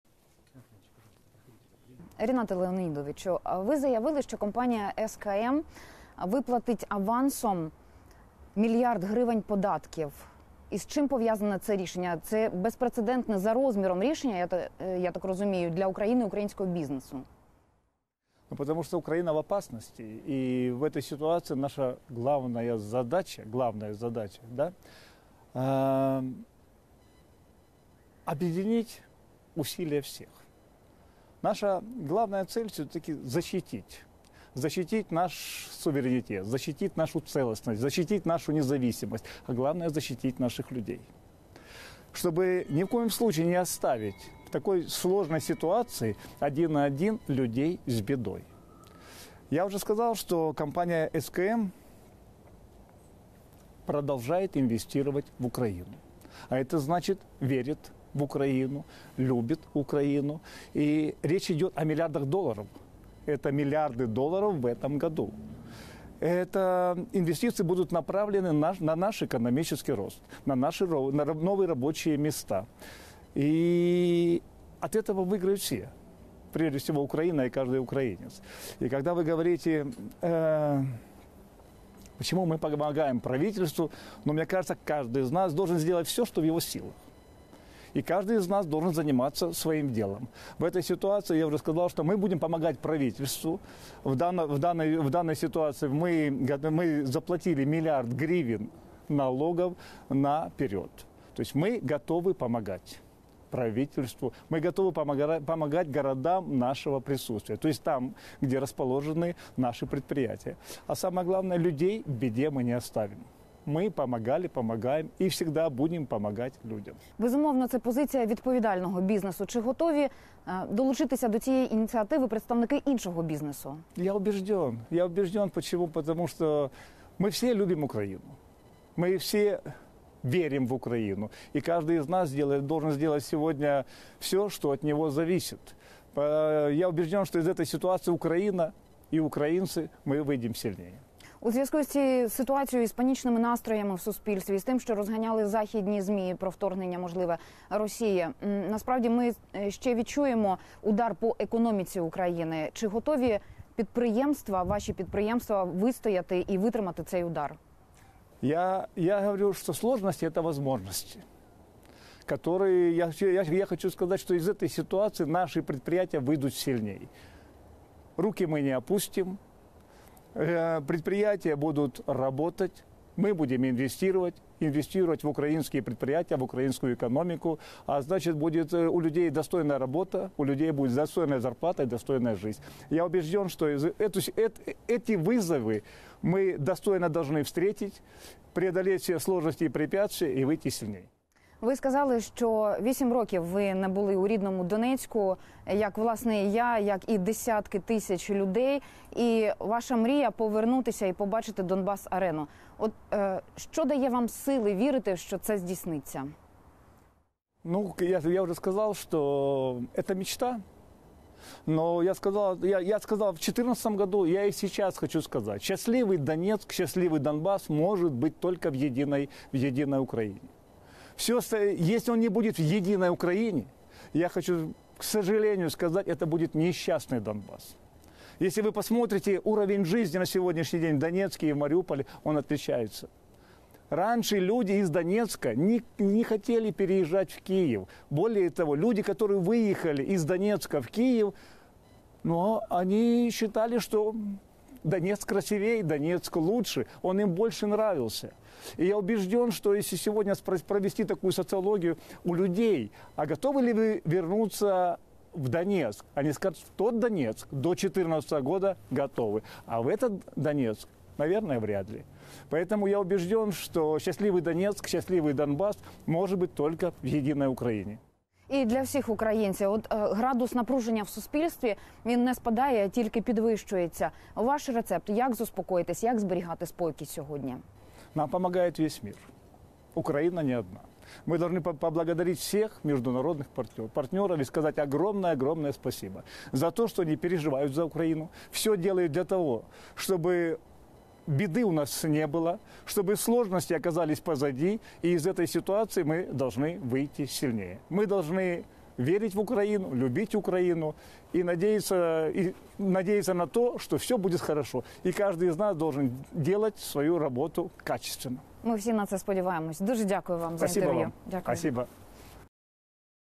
Мільярд гривень податків авансом сплатить компанія SCM
Компанія SCM продовжить інвестувати в Україну, заявив Рінат Ахметов в свіжому інтерв’ю.

Бізнесмен повідомив, що у теперішніх надскладних умовах кожен має робити все, аби допомогти державі. Відтак бізнес й надалі інвестуватиме в розвиток України.

Це допоможе подолати економічні складнощі та не залишити громадян сам-на-сам із бідою, наголосив Рінат Ахметов.